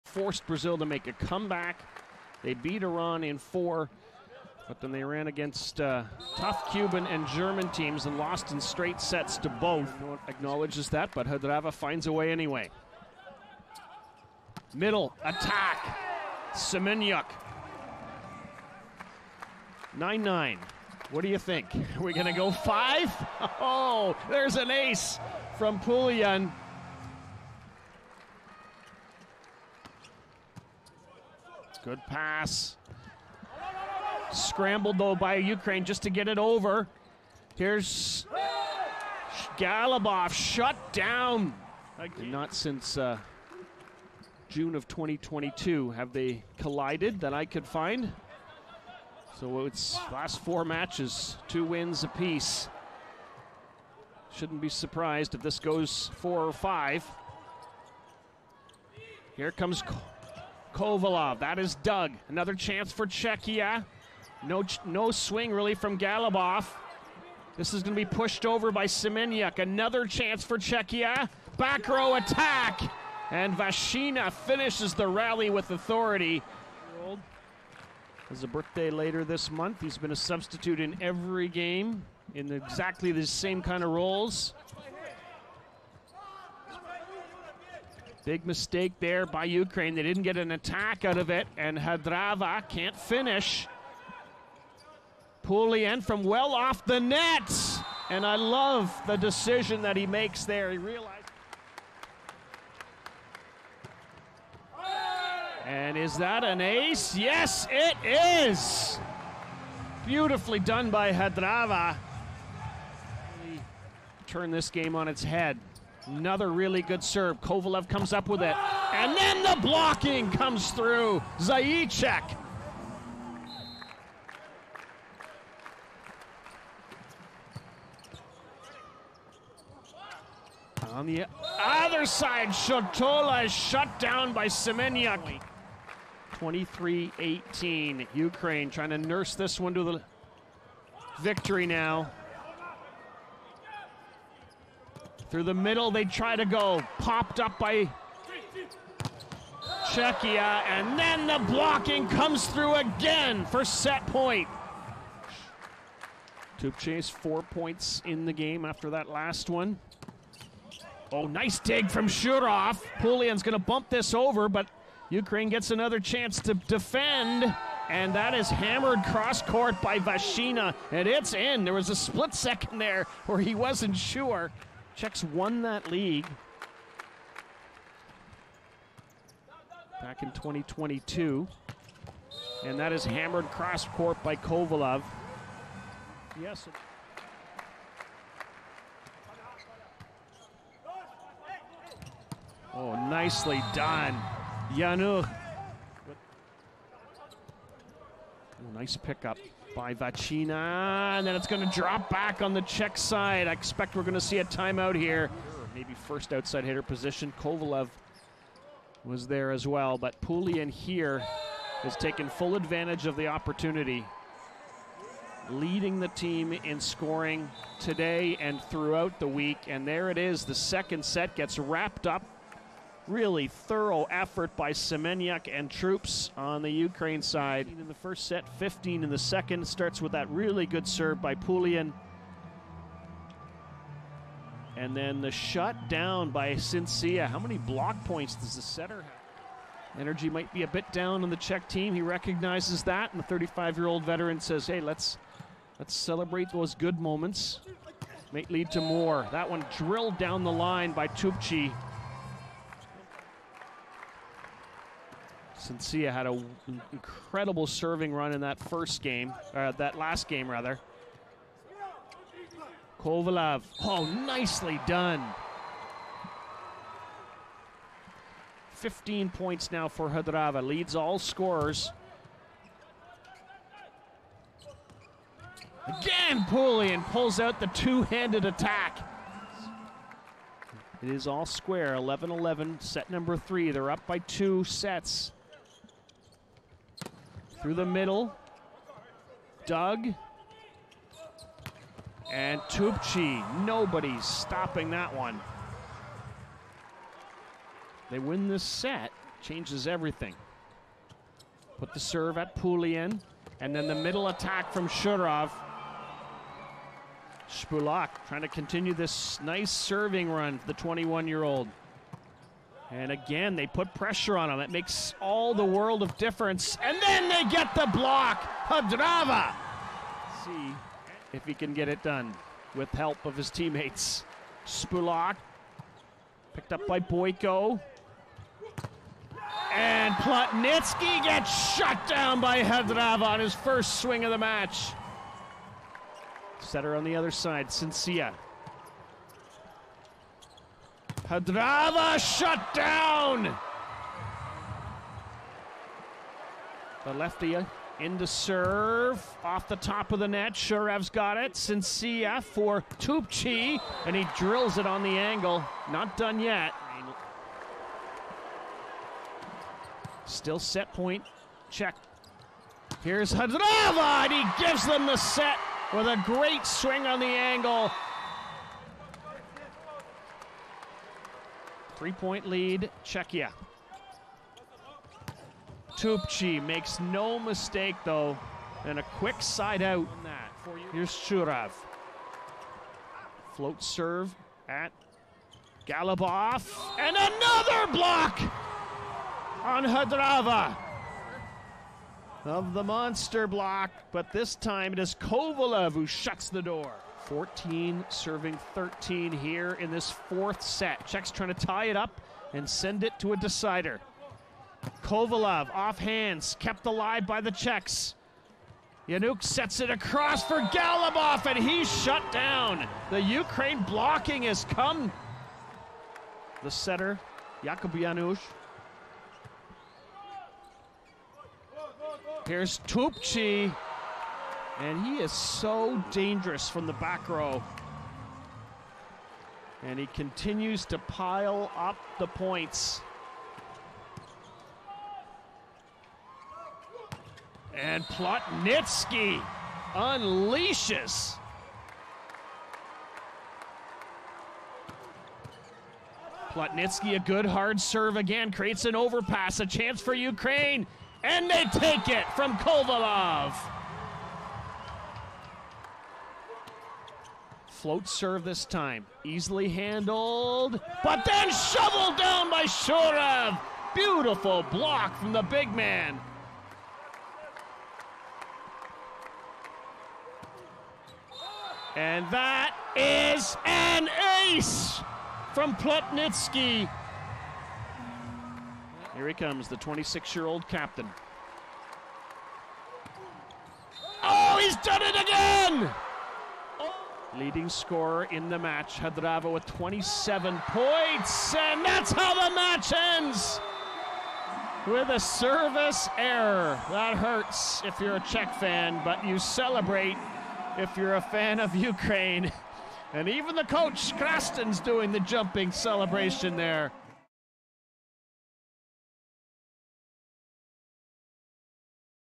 🇺🇦 UKR vs. 🇨🇿 CZE - Highlights | Men's OQT 2023
Watch the men's highlights between Ukraine and Czechia from (Pool) of the Olympic Qualification 2023 in Rio de Janeiro (Brazil)!

LOVE BEACH VOLLEYBALL?